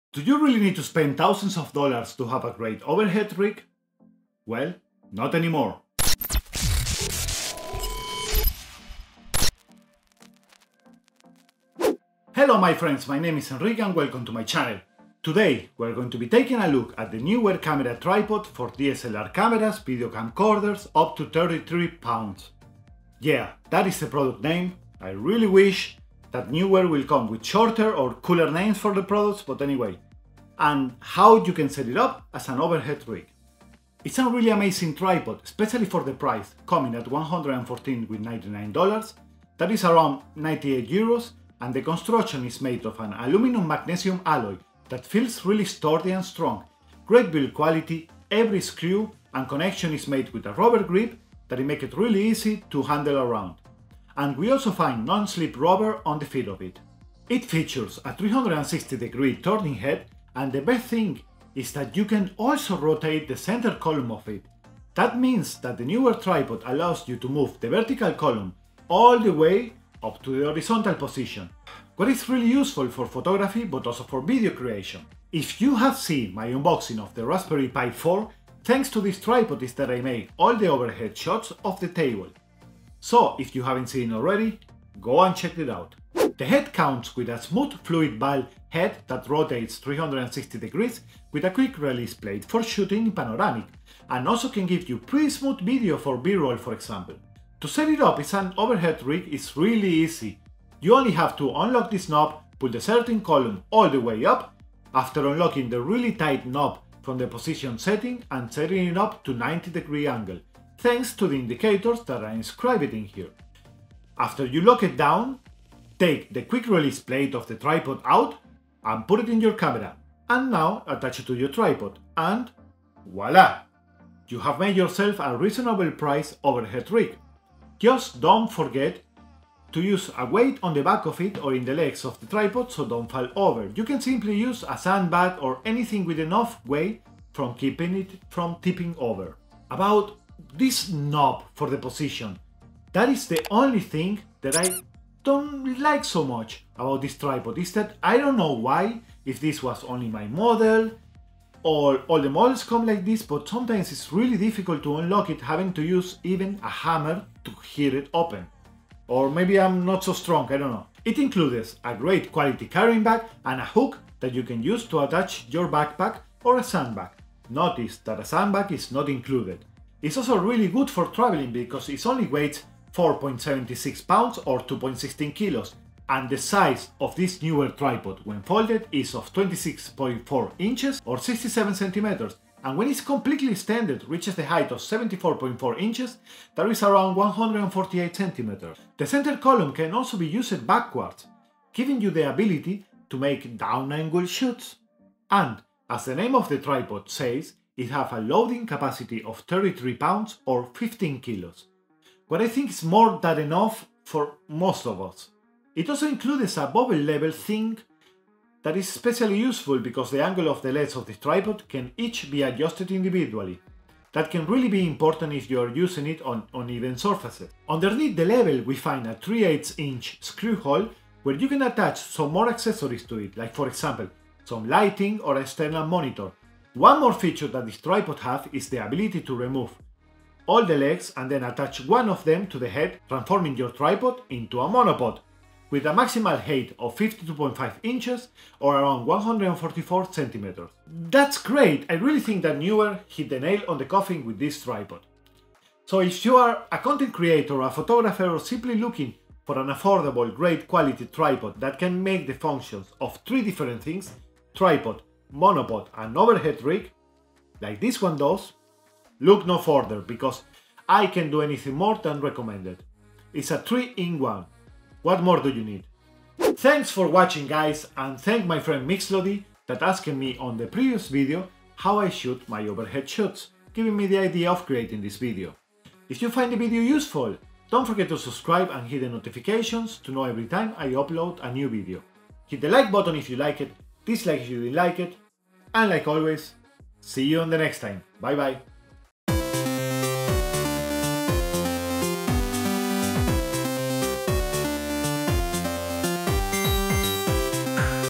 CHEAP Overhead Camera Rig | Neewer N284+Go Tripod Review
A cheap overhead camera rig? Today i review the Neewer N284+Go Tripod, that apart of being an budget tripod  you dont only get a tripod, but can also turn into a really affordable overhead camera rig with his horizontal arm head for shooting videos and even a monopod.

So if you are looking to create videos and tutorials using the overhead video format for example on a table, i think that this tripod from neewer can really help specially for the price that is really affordable.